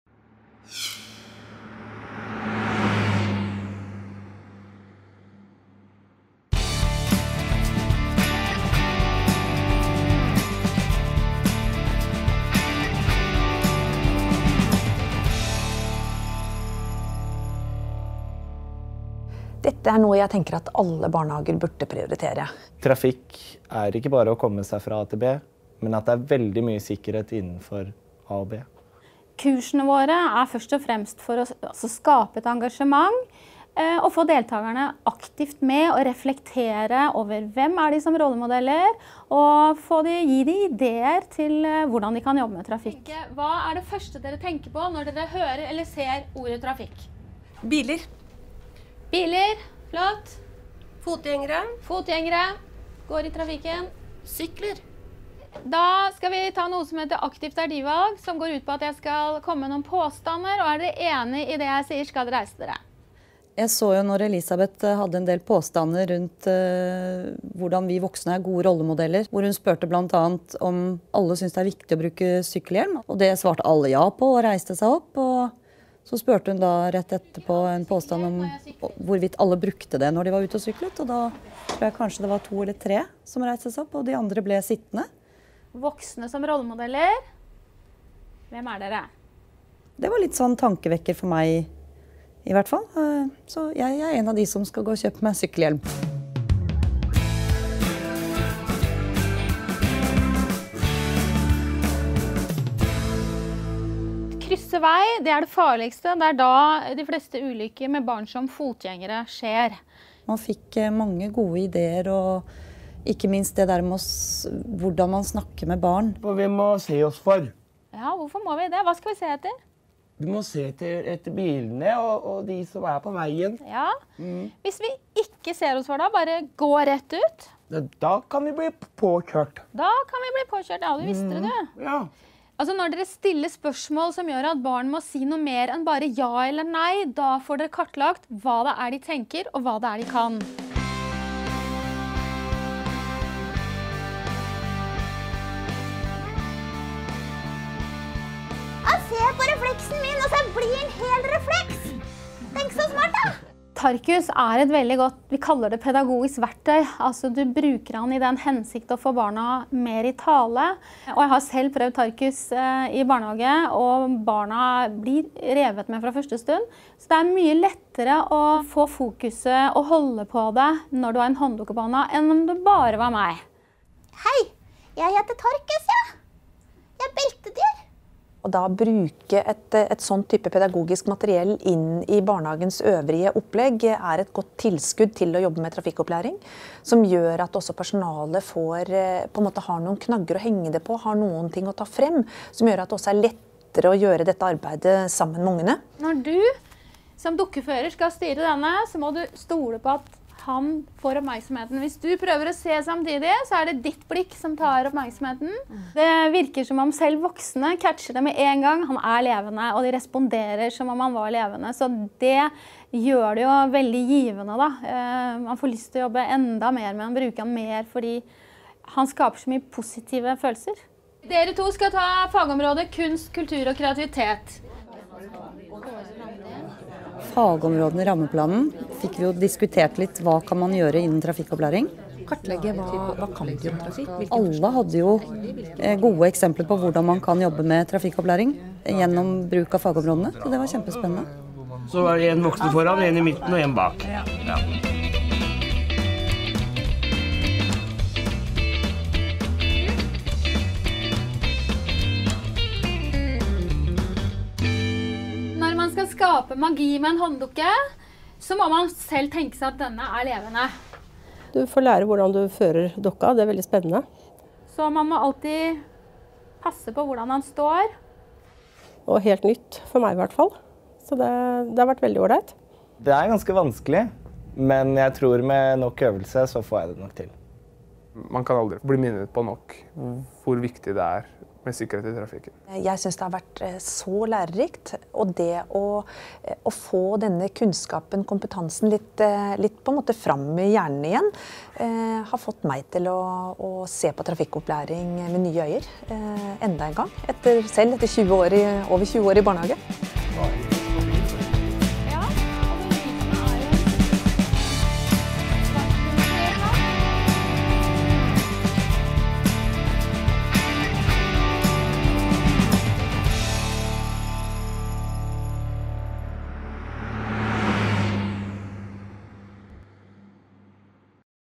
barnehagekurs
Filmen gir et innblikk i kursene Trygg Trafikk holder for barnehageansatte. Kursene gir mange nyttige tips og gode ideer til hvordan trafikksikkerhet og trafikkopplæring kan ivaretas i barnehagen. Beltedyret Tarkus er også med!